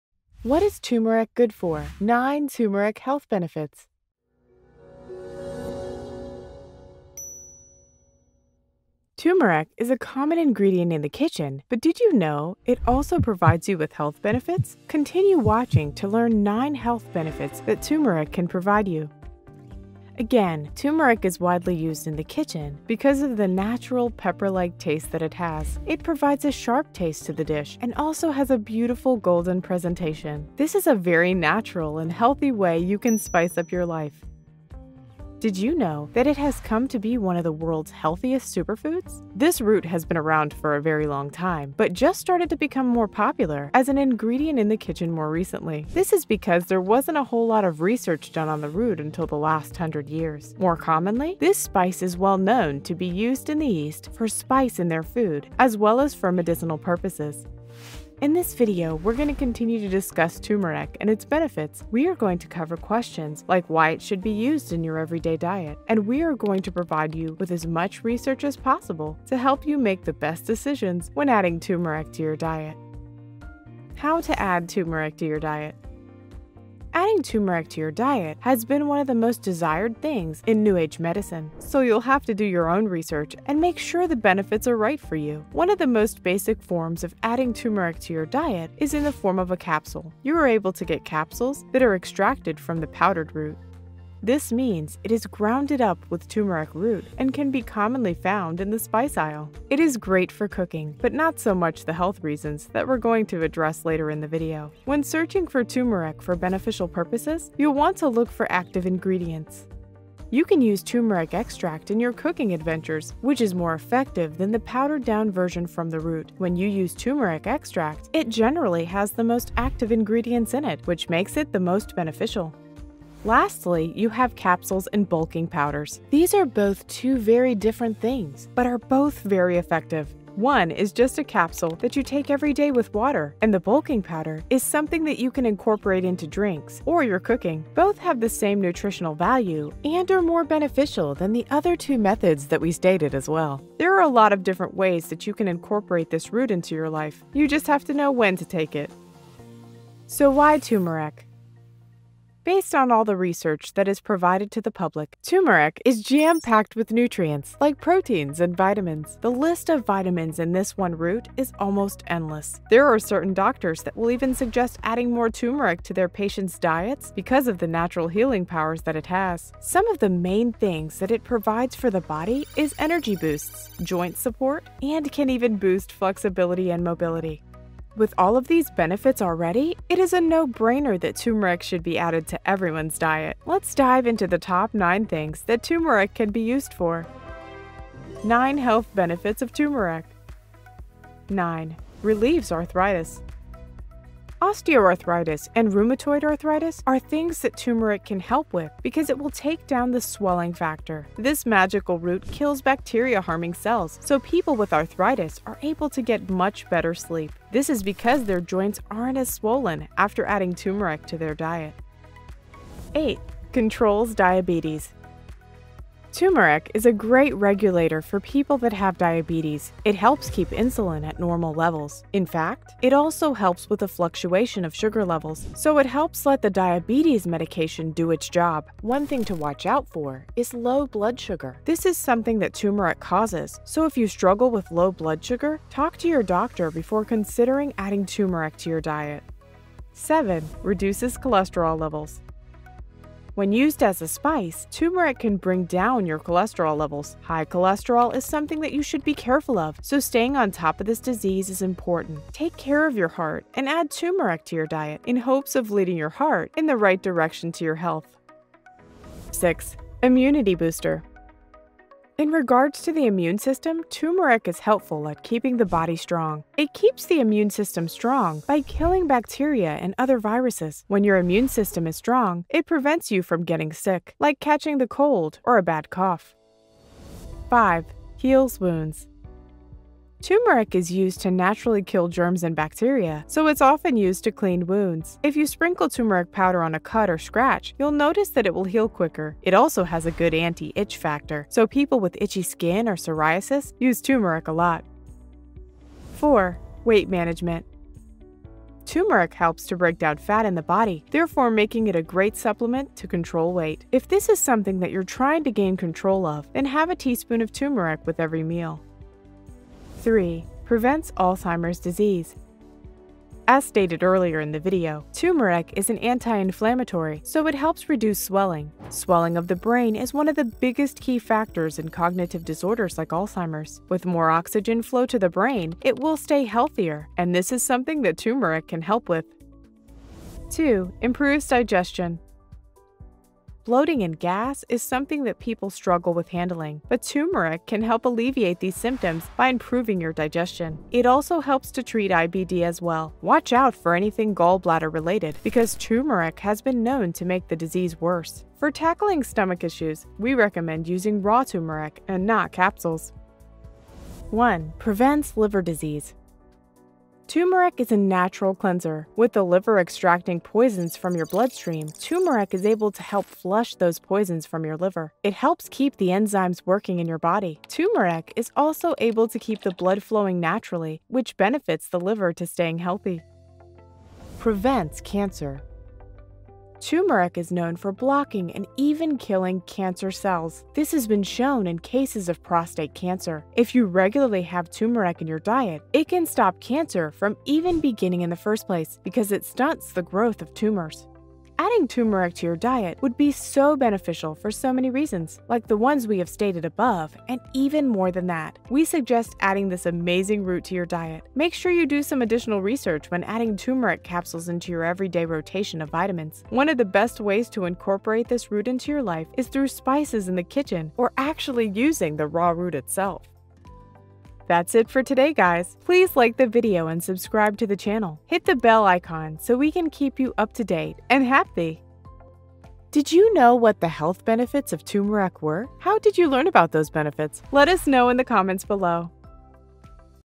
Health Benefits of Turmeric | What Turmeric is Good For
Turmeric is widely used in the kitchen because of the natural pepper-like taste that it has. It provides a sharp taste to the dish and also has a beautiful golden presentation. This is a very natural and healthy way that you can spice up your life.

Did you know that it has come to be one of the world's healthiest superfoods? This root has been around for a very long time, but just started to become more popular as an ingredient in the kitchen more recently. This is because there wasn’t a whole lot of research done on the root until the last hundred years. More commonly, this spice is well known to be used in the East for spice in their food as well as for medicinal purposes.

In this video we’re going to continue to discuss turmeric and its benefits. We are going to cover questions like why it should be used in your everyday diet. And we are going to provide you with as much research as possible to help you make the best decisions when adding turmeric to your diet.

Timestamps:
Intro 0:00
How Consume Turmeric 1:13
Relieves Arthritis 3:11
Controls Diabetes 3:32
Reduces Cholesterol Levels  3:59
Immunity Booster 4:18
Heals Wounds 4:37
Weight Management 4:46
Prevents Alzheimer's Disease 5:10
Improves Digestion 5:30
Prevents liver disease 5:54
Bonus Fact 6:14

Relieves Arthritis. Osteoarthritis and rheumatoid arthritis are other things that turmeric can help with because it will take down the swelling factor. This magical root kills bacteria harming cells, so people with arthritis are able to get much better sleep. This is because their joints aren’t as swollen after adding turmeric to their diet.

Controls Diabetes. Turmeric is a great regulator for people that have diabetes. It helps keep insulin at normal levels. In fact, it also helps with the fluctuation of sugar levels, so it helps let the diabetes medication do its job. One thing to watch out for is low blood sugar. This is something that turmeric causes, so if you struggle with low blood sugar talk to your doctor before considering adding turmeric to your diet...

Reduces Cholesterol Levels . When used as a spice, turmeric can bring down your cholesterol levels. High cholesterol is something that you should be careful of, so staying on top of this disease is important. Take care of your heart and add turmeric to your diet in hopes of leading your heart in the right direction to health...

Prevents Alzheimer's Disease. As stated earlier in the video, turmeric is an anti-inflammatory, so it helps reduce swelling. Swelling of the brain is one of the biggest key factors in cognitive disorders, like Alzheimers. With more oxygen flow to the brain, it will stay healthier and this is something that turmeric can help with...

Prevents liver disease. Turmeric is a natural cleanser. With the liver extracting poisons from your bloodstream, turmeric is able to help flush those poisons from your liver. It helps keep the enzymes working in your body. Turmeric also is able to keep the blood flowing naturally which benefits the liver to staying healthy...[Please watch the video for the full list of Turmeric Health Benefits]